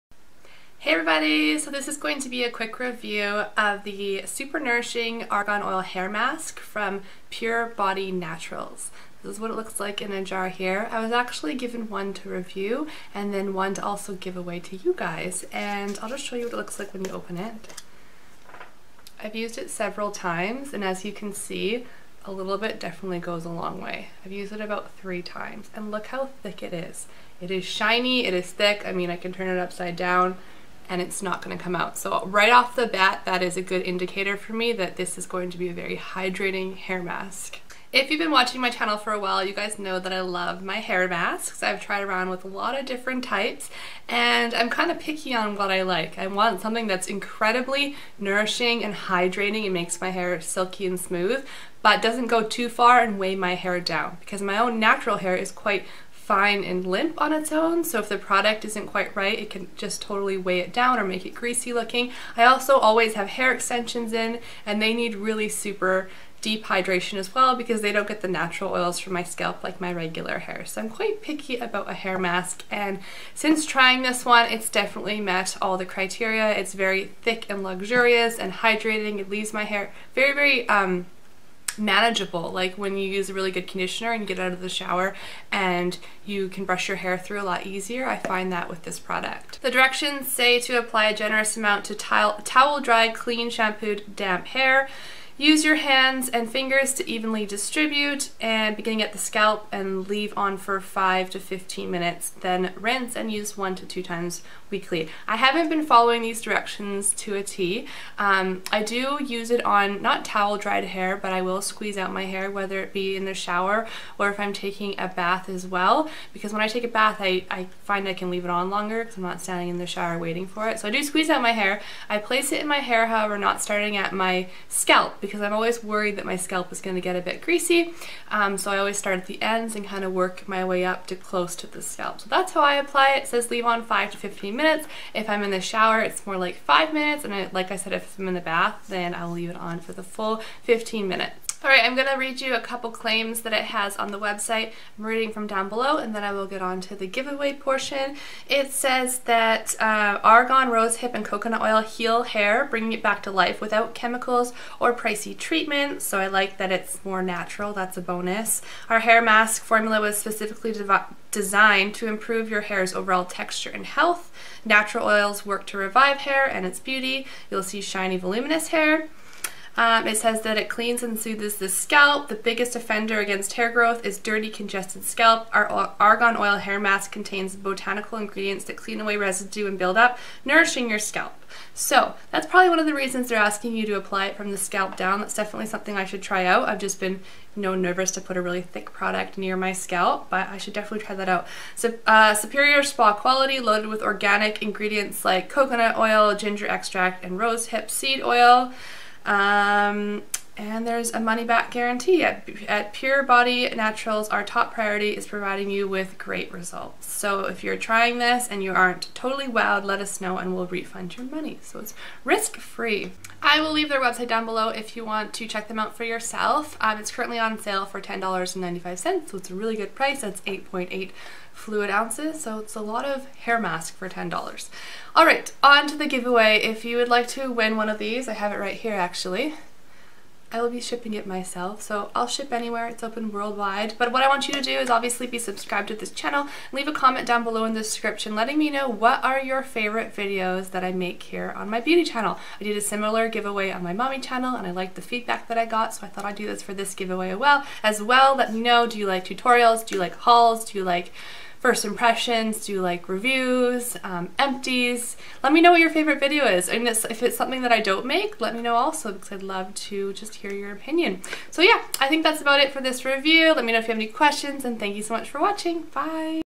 Argan Oil Hair Mask Review & Giveaway! - Pure Body Naturals
Hey everyone! This is my review of the Pure Body Naturals Argan Oil Hair Mask. I will leave the giveaway rules and link to purchase down below :)

Argan Oil Mask

2. Leave me a comment telling me your favorite type of video that I make. Or, request a video I've never made before :)